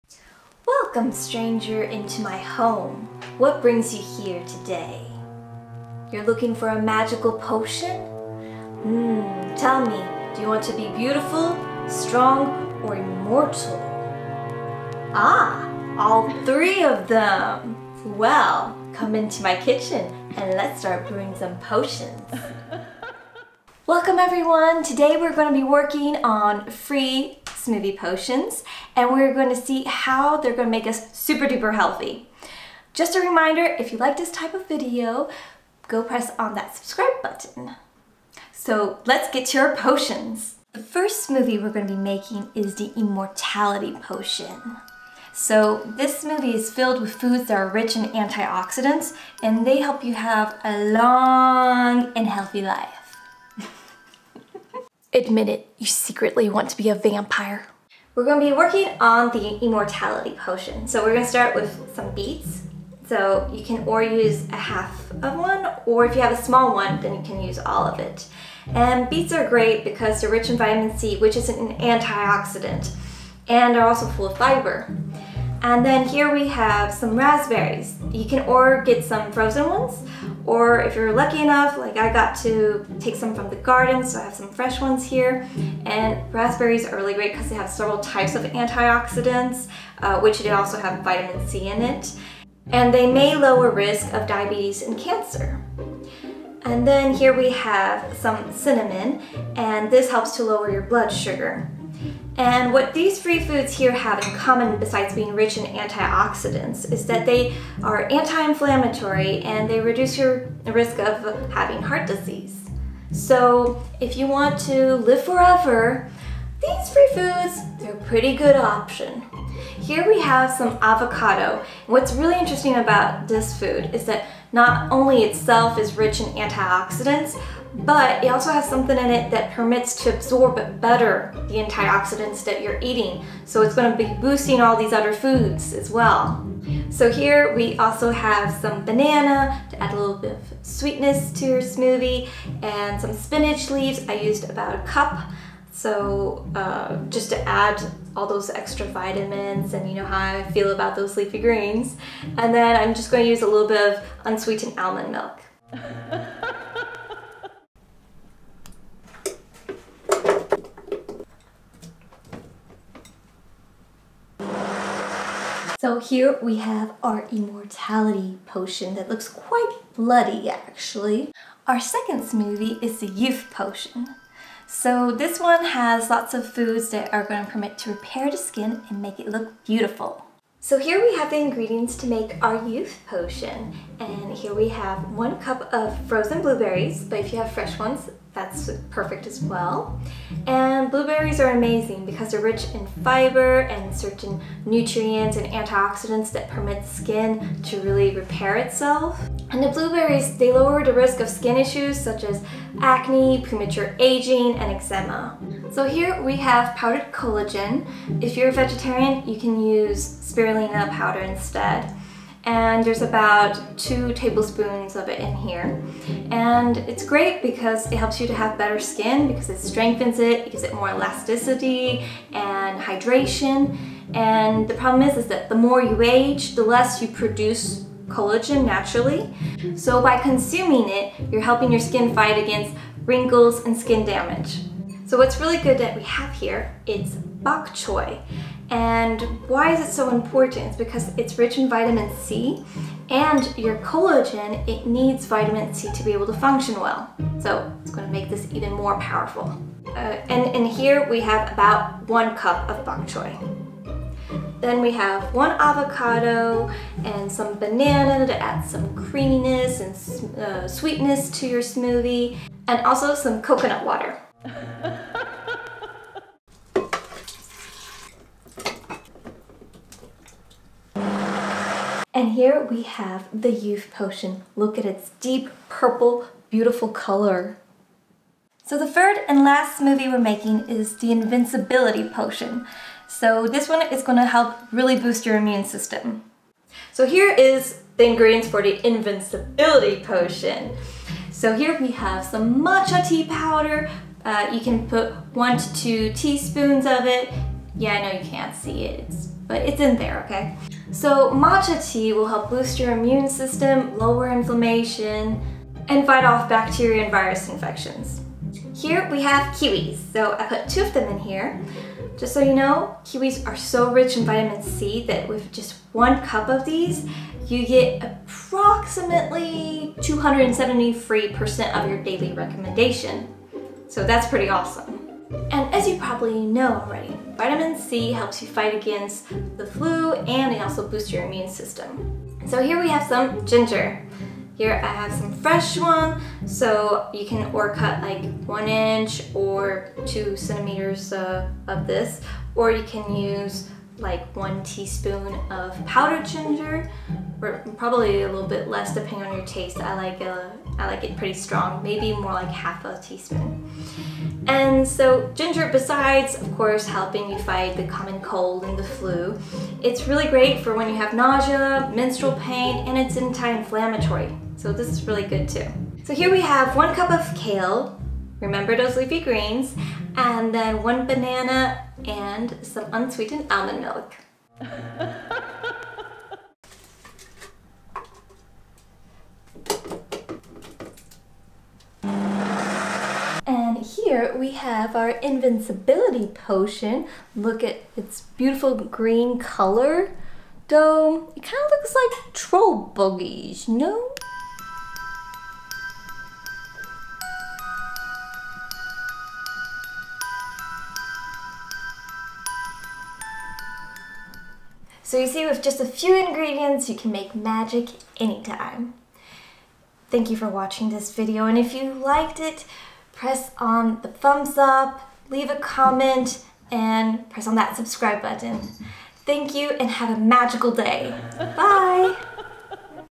3 healthy smoothie recipes (special Halloween potions)
Discover 3 healthy smoothies "potions" to drink. You will learn how to make them to boost your immunity, have more beautiful skin and to live a long and healthy life!... What I meant was that you will find the "immortality potion", the "youth potion" and the "invincibility potion"!